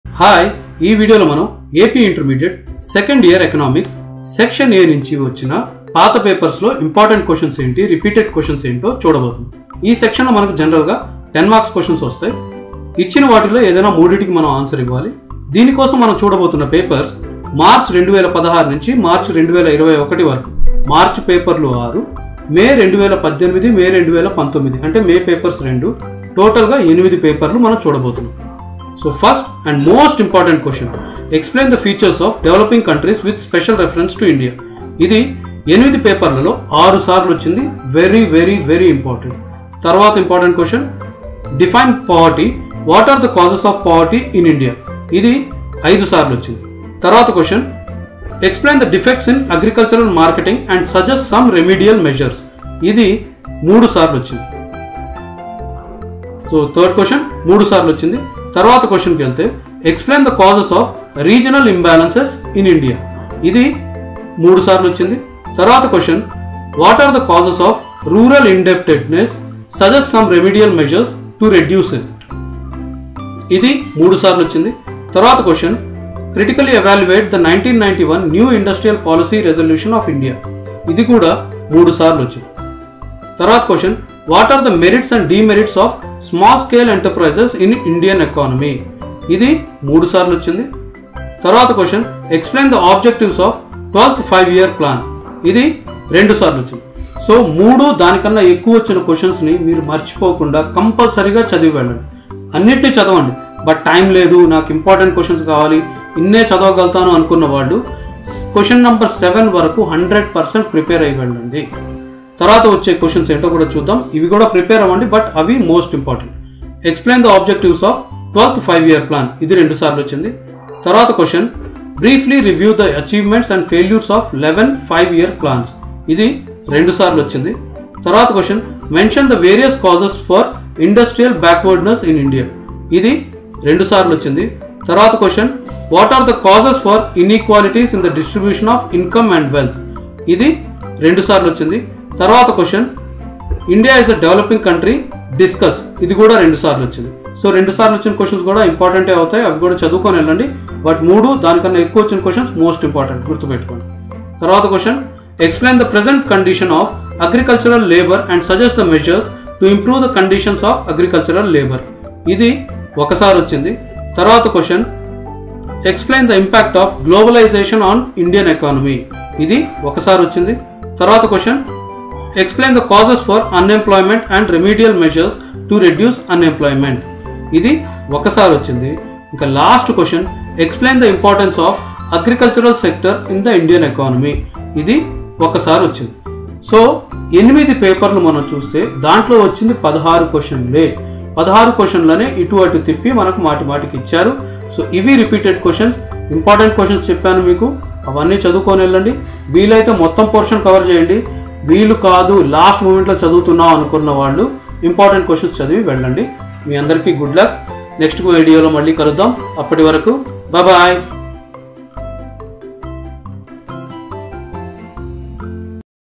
AP Inter 2nd Year Economics Repeated & Important Questions from Section A 10 marks questions
All questions asked from Section A of Economics Paper in AP inter 2nd year between 2016 to 2021 Papers.